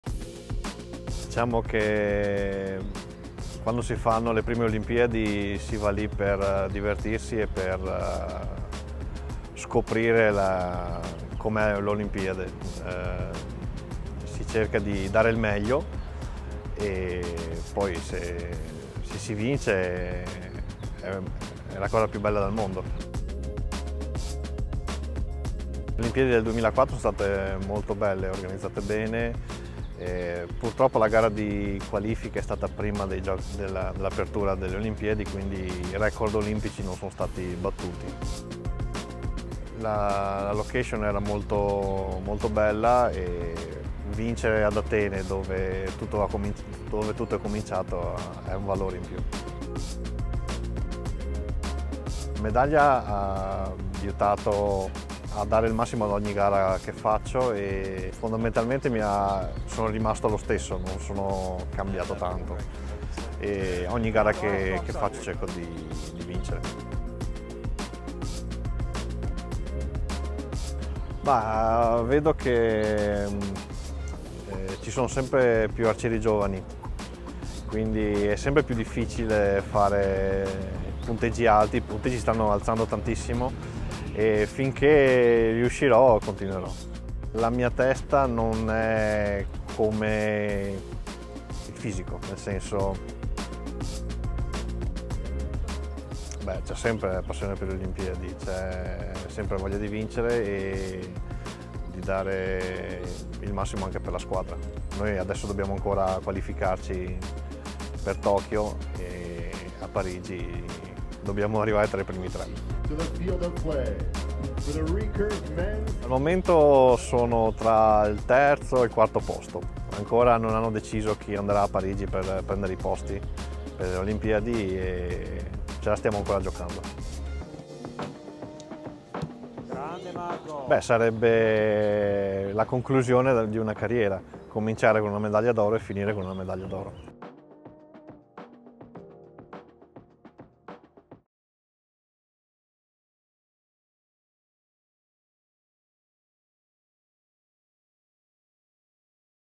Marco Galiazzo started his career with Olympic gold and he wants to finish it with Olympic gold
Marco Galiazzo won the Olympic Games at Athens 2004 – in his first appearance at the Games. Seventeen years later, he still has hopes of winning a second gold at the Olympics.